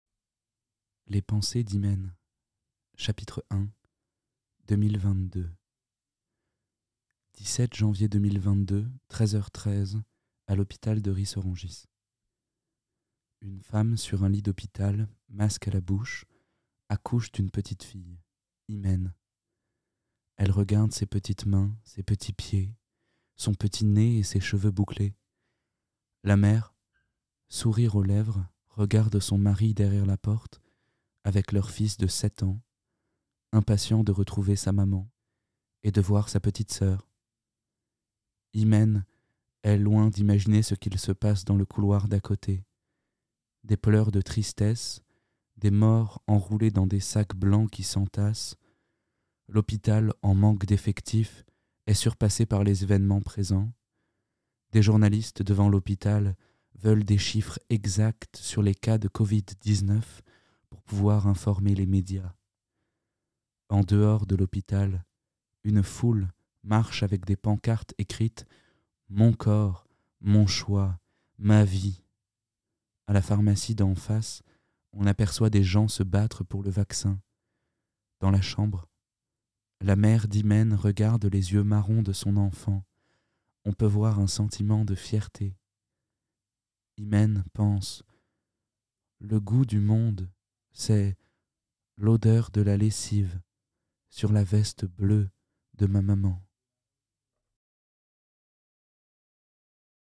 Les pensées d'Imen chapitre 1 - Prix jeunesse du Muséum littéraire, lycée Pierre Mendès France (91)
"Les pensées d'Imen" est un des trois textes lauréats de la 1ère édition du Prix jeunesse du Muséum littéraire - Nouvelles du futur organisé par le Muséum national d'histoire naturelle et la Maison des écrivains et de la littérature, avec la collaboration des académies de Créteil, Paris et Versailles.  Accompagnés par l’écrivain Adèle Gascuel, les élèves de la classe de 1ere Agora du lycée Pierre Mendès France à Ris-Orangis (91 - académie de Versailles) ont remporté le prix de la création littéraire. Lecture par Simon Salomon, comédien.